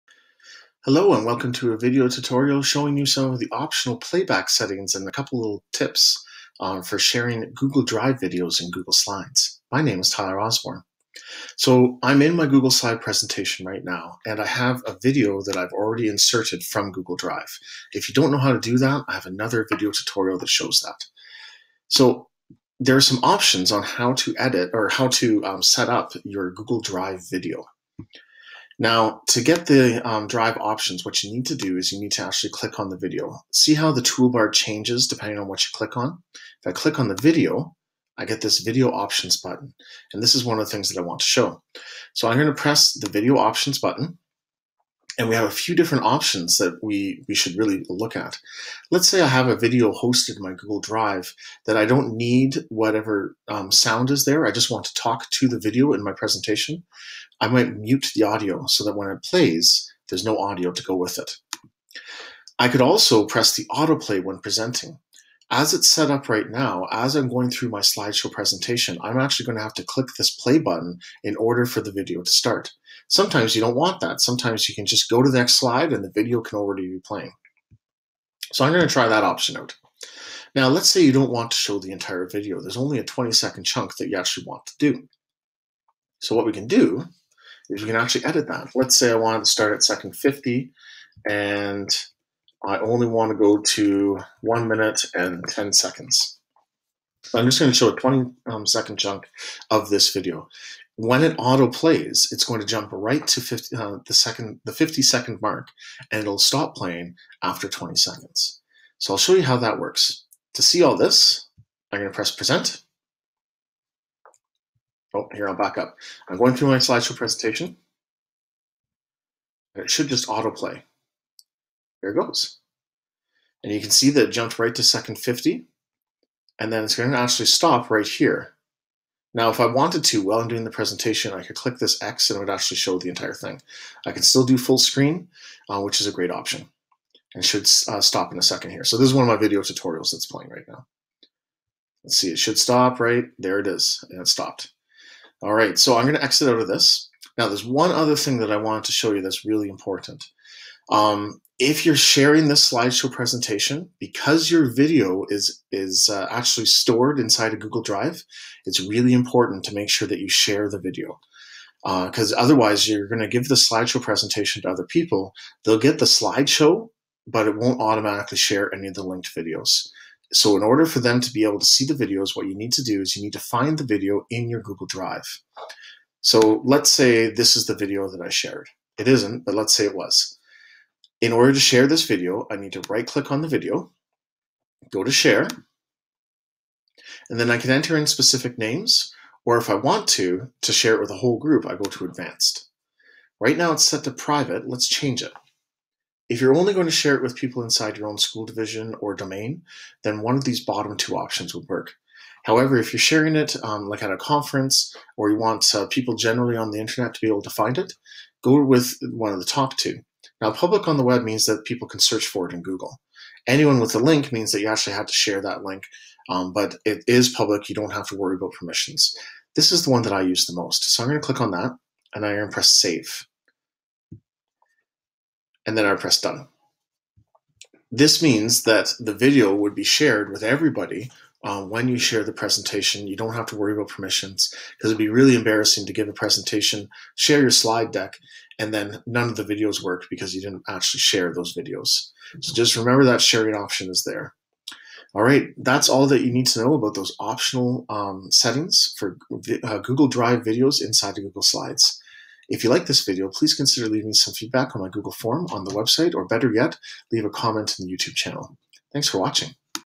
Optional playback settings for Google Drive videos in Google Slides (plus sharing tips)
This short tutorial demonstrates optional playback settings that allow you to mute the video, autoplay, and choose to only show specific sections of a longer video. It also gives tips for sharing videos inside of Google Slide decks.